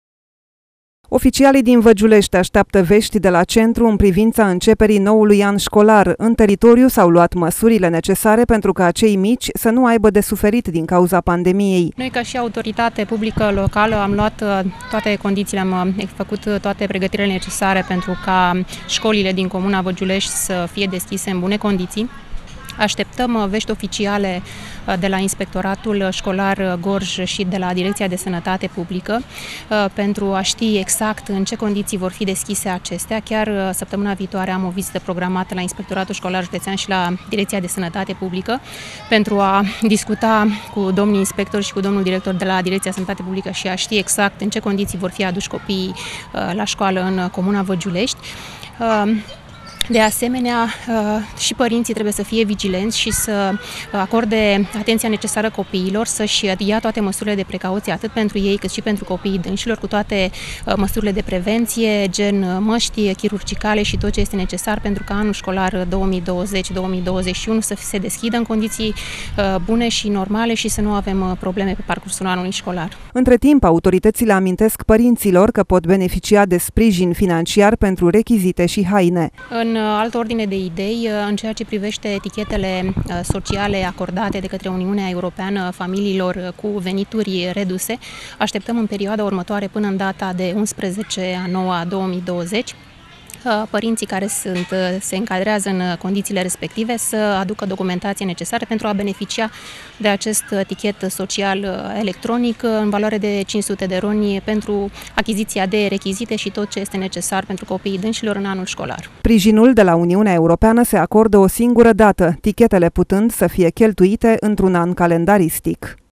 ȘCOLILE DIN VĂGIULEȘTI SUNT PREGĂTITE
Scolile din Vagiulesti sunt pregatite sa-si primeasca elevii. Autoritatile locale au luat masuri necesare pentru ca cei mici sa fie primiti in siguranta